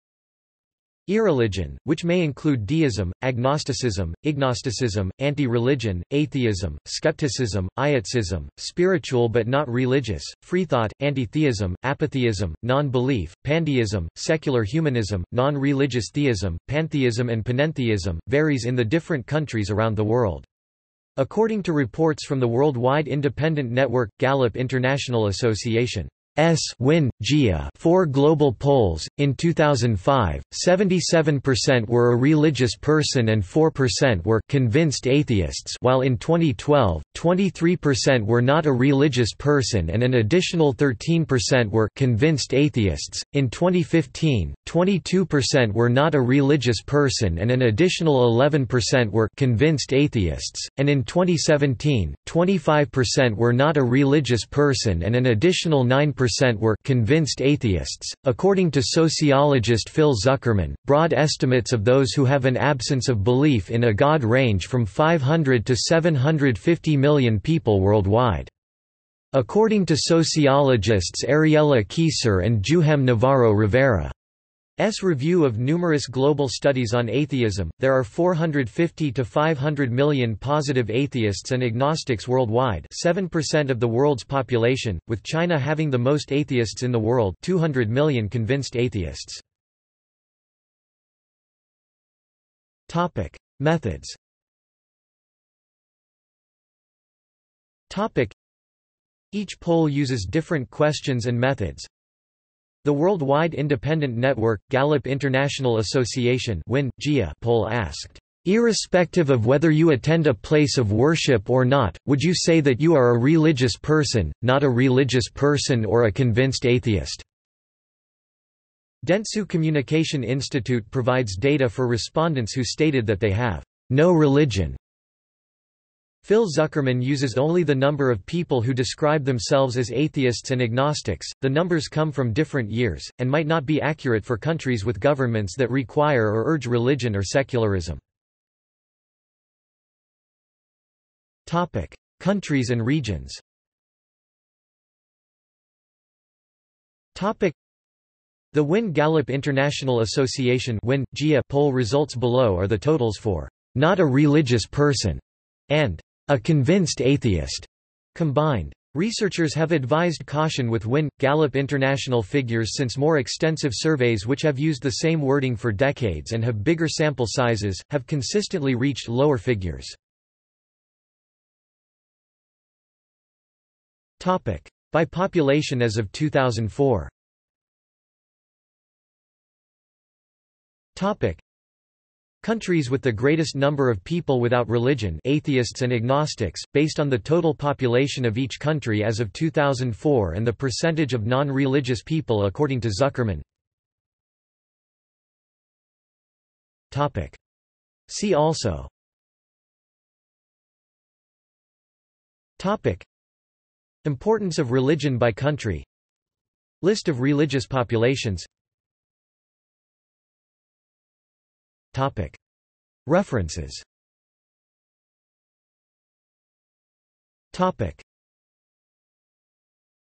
List of countries by irreligion | Wikipedia audio article
This is an audio version of the Wikipedia Article:
List of countries by irreligion

SUMMARY
Irreligion, which may include deism, agnosticism, ignosticism, anti-religion, atheism, skepticism, ietsism, spiritual but not religious, freethought, anti-theism, apatheism, non-belief, pandeism, secular humanism, non-religious theism, pantheism and panentheism, varies in the different countries around the world. According to reports from the Worldwide Independent Network/Gallup International Association's (WIN/GIA) four global polls: in 2005, 77% were a religious person and 4% were "convinced atheists" while in 2012, 23% were not a religious person and an additional 13% were "convinced atheists"; in 2015, 22% were not a religious person and an additional 11% were "convinced atheists"; and in 2017, 25% were not a religious person and an additional 9% were "convinced atheists".According to sociologist Phil Zuckerman, broad estimates of those who have an absence of belief in a God range from 500 to 750 million people worldwide. According to sociologists Ariela Keysar and Juhem Navarro-Rivera's review of numerous global studies on atheism, there are 450 to 500 million positive atheists and agnostics worldwide (7% of the world's population), with China having the most atheists in the world (200 million convinced atheists).